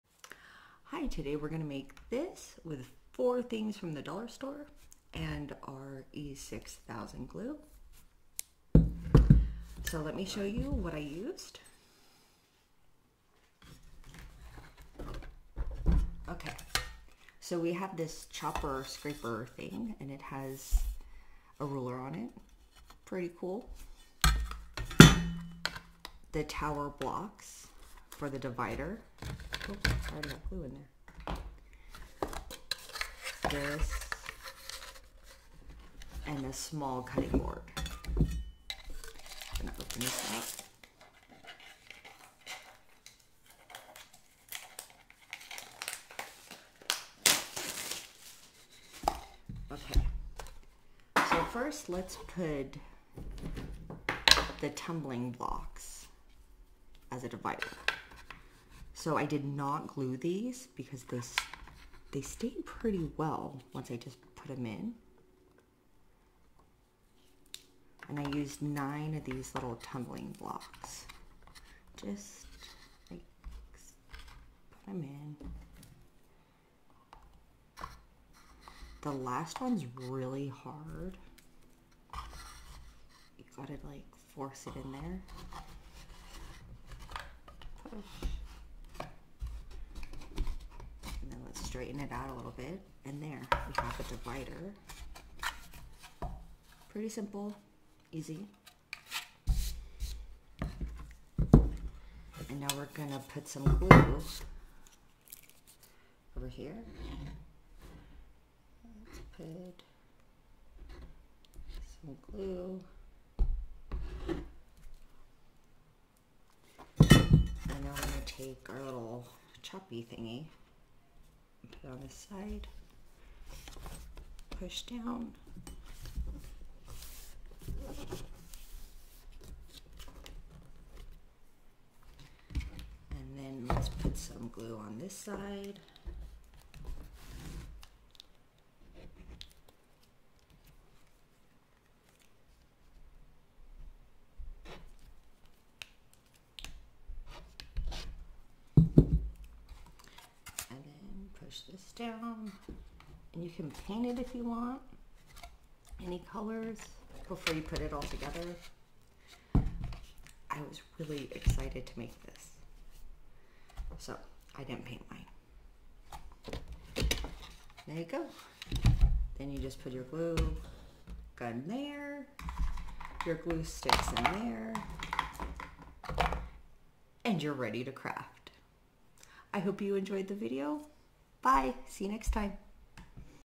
How-To: Hot Glue Gun Stand and Glue Stick Holder
In this video I will show you how to make a hot glue gun stand and glue stick holder using 4 items found at Dollar Tree and some E6000 glue:

All items found at Dollar Tree (unless they run out):

Small Cutting Board
Pastry Scraper
Wooden Tumbling Blocks Game
Small Wood Crate